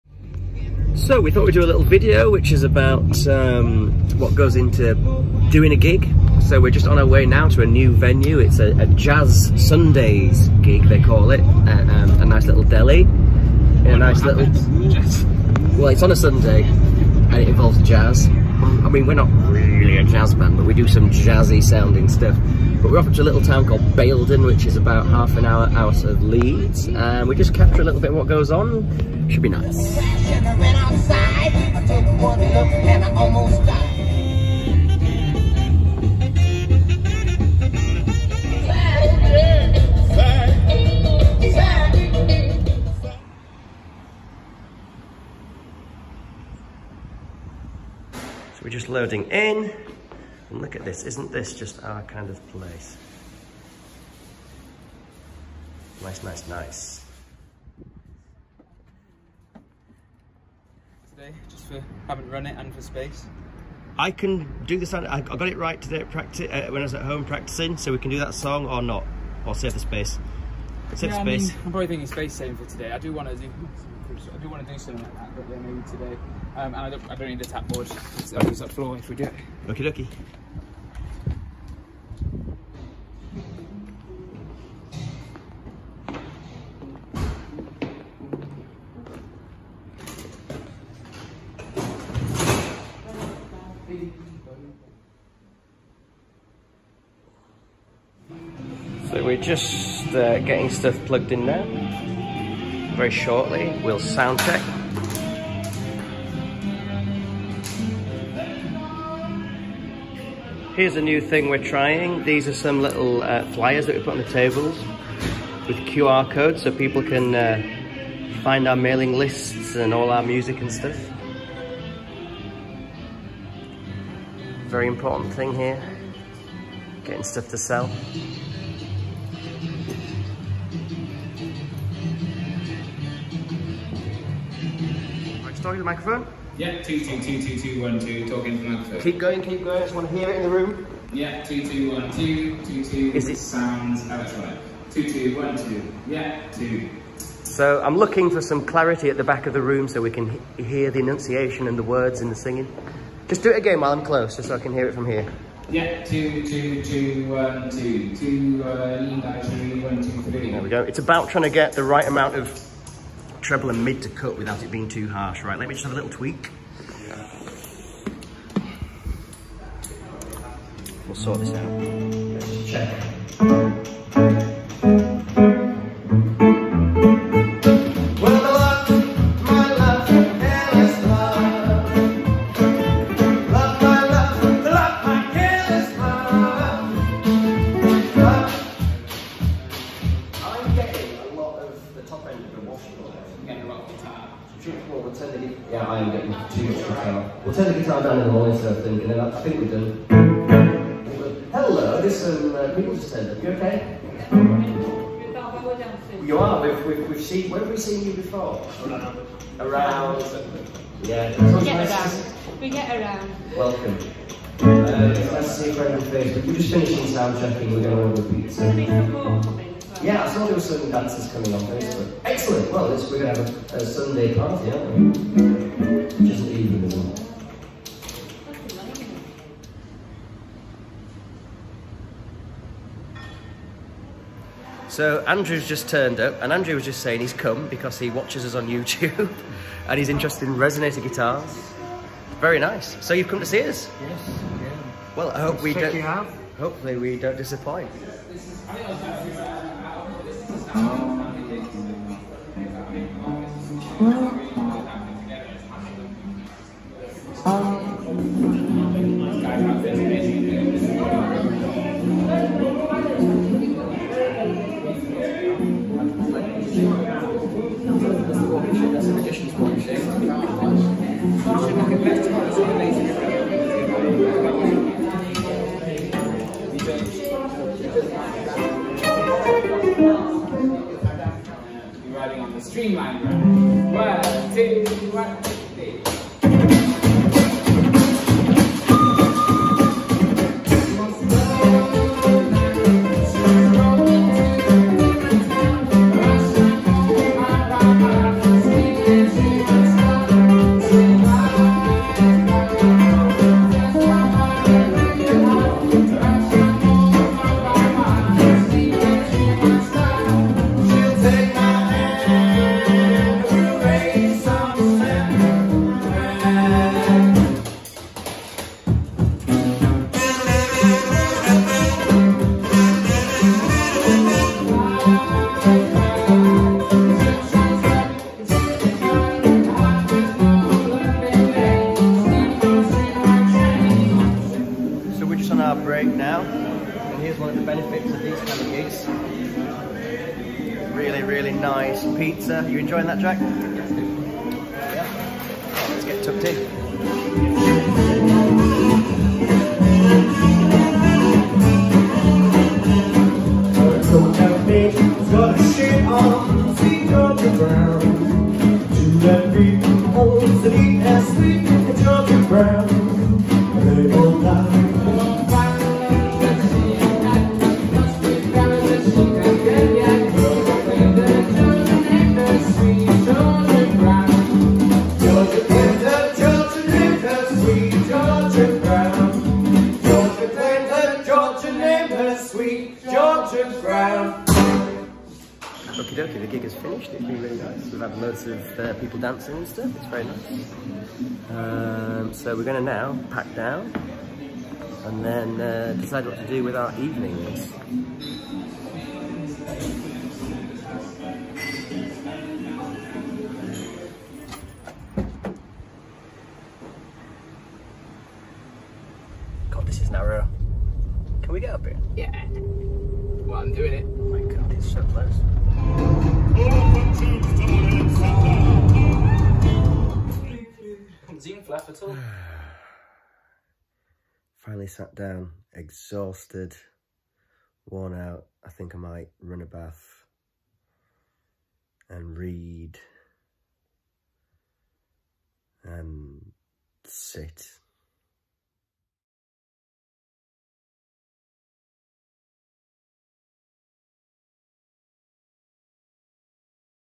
Typical Gig Set Up - The Washboard Resonators - Pickles Deli, Baildon
We go set up for a gig and share the experience for you as a little bonus video this week.

We play ‘Jazz Sundays’ at Pickles Deli, Baildon, West Yorkshire.

We set up our washboard and resonator guitars for an afternoon of blues, jazz, ragtime and hokum!

The best ways to support us;

Personal links;